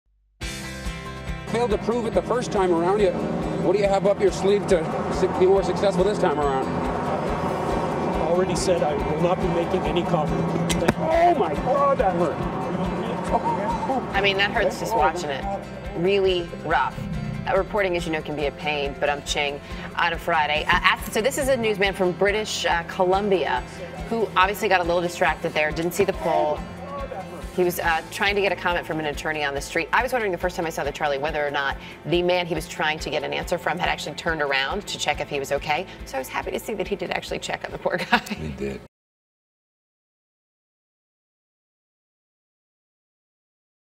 News blooper: Reporter walks into pole
Charlie Rose and Erica Hill take a look at a news blooper that shows a reporter walking into a pole while questioning an attorney.